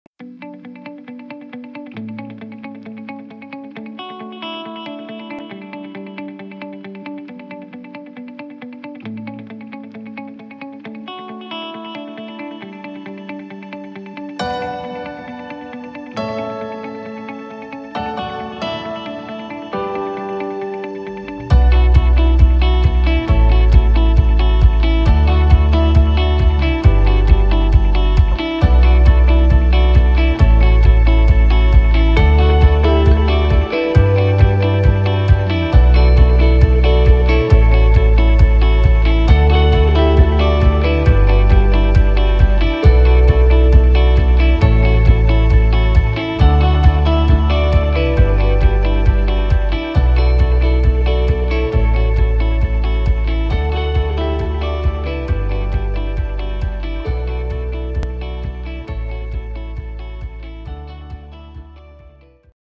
ZIFIX čistenie fasád
Čistenie fasád, striech, dlažieb. Práce vo výškach a čistenie všetkých povrchov.